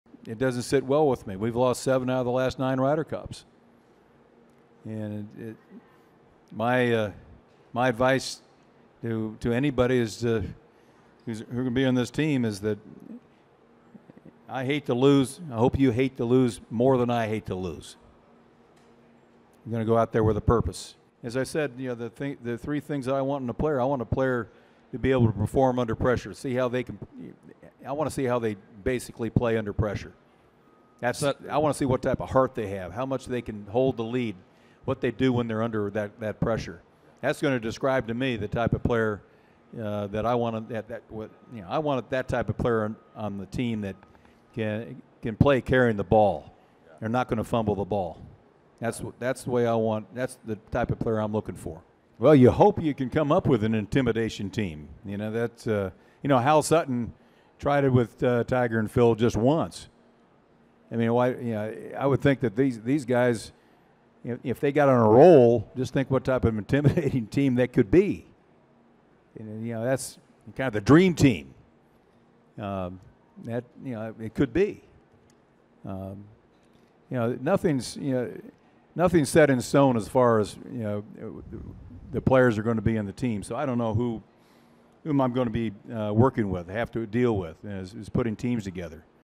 USA Ryder Cup captain Watson wants more from players
2014 USA Ryder Cup captain wants his players to stop the rot when they face Europe at Gleneagles next September.